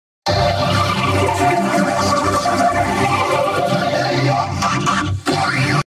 [REVIVED EFFECT] Klasky Csupo in Bormulator G0
HTM:
Video Fx
HSL Adjust Add To Hue 0.150
Mirror Preset Reflect Bottom
Color Curves Blues and Yellows
NB ME Wiggle Rate 25 Strength 100 Drift 15
Edges Stretch Orentation Horizonlat

Audio Fx Is Unknow